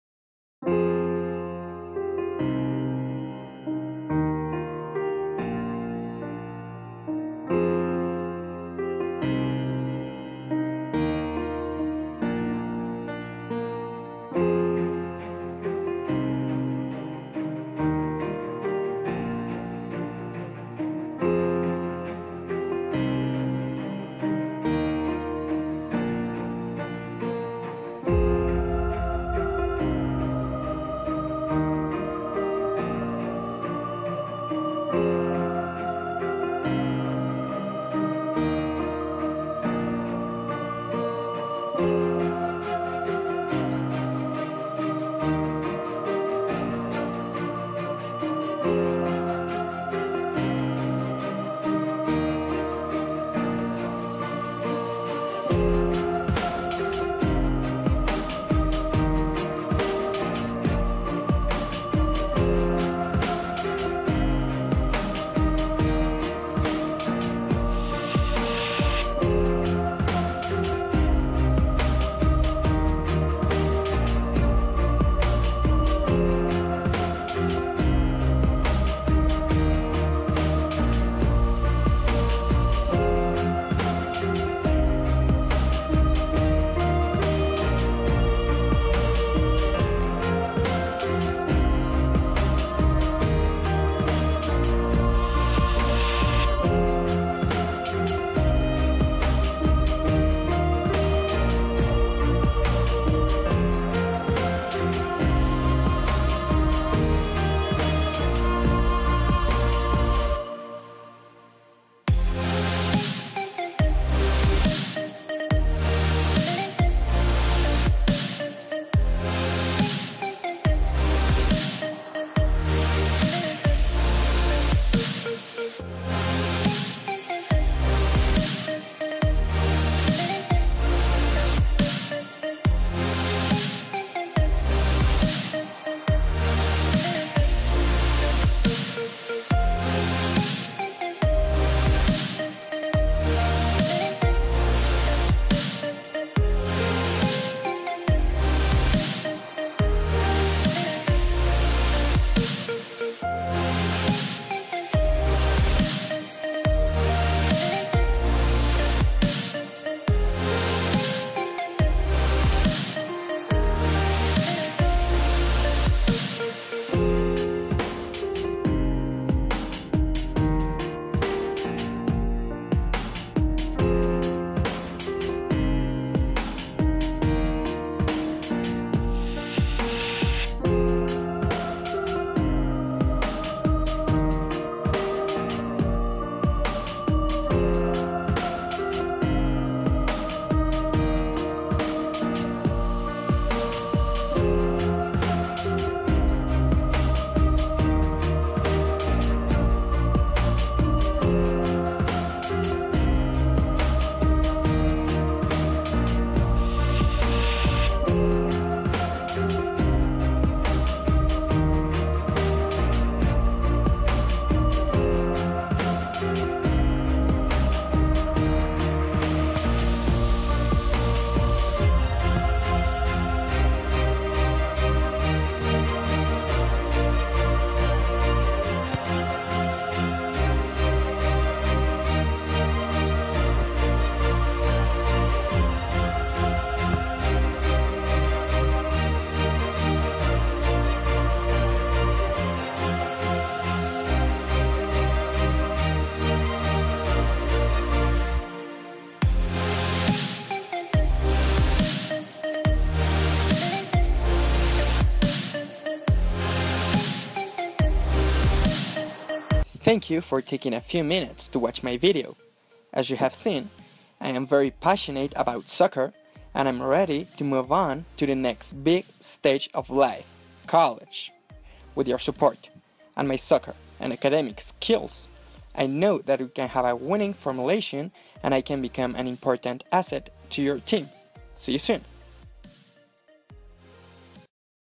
Lus de la Torre - Mexican Student/Soccer player
I want to go to the USA and Study in a well-respected University supported by my soccer skills and experience from countries such as Mexico, Spain and Brazil.

SAT – 1120 (CR 520, Math 600 + Writing 530).

TOEFL - 89